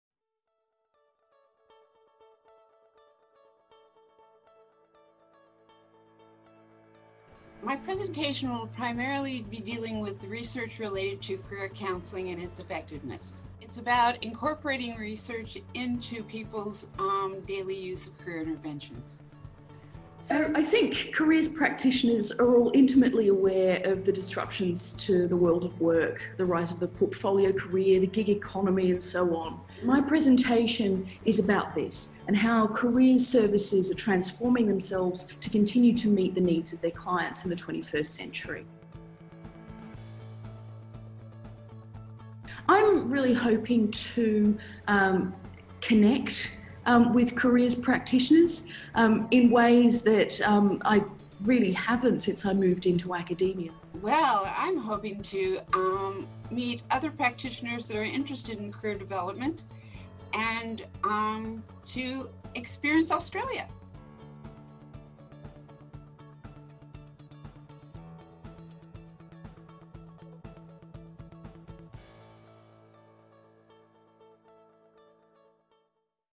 2018 Keynote Speaker Promotional Video
The CDAA2018 National Conference will be held in Hobart from 2-4 May.

We spoke to two of the keynote speakers - Dr Susan C. Whiston and Professor Ruth Bridgstock - about their presentation topics.